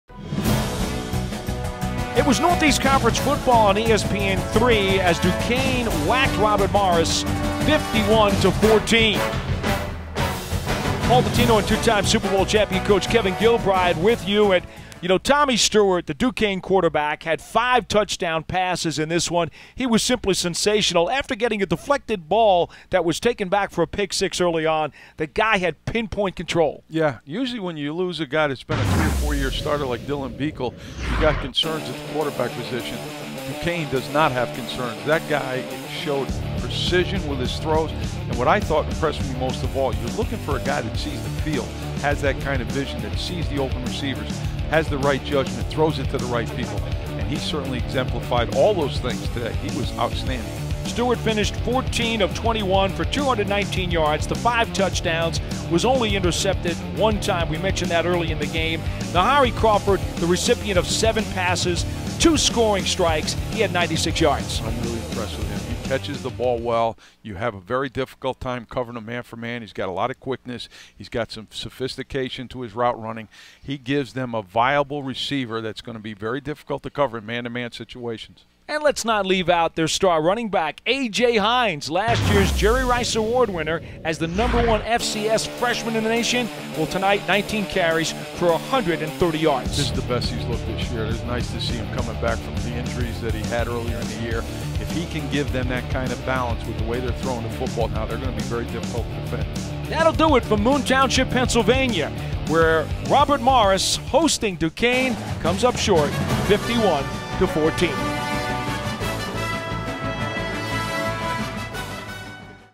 NECFB - Duquesne defeats Robert Morris, 51-14
In our NECFB game of the week on ESPN 3, Duquesne took down keystone state rival Robert Morris 51-14. Dukes' QB Tommy Stuart threw for 219 yards and five touchdowns in the win.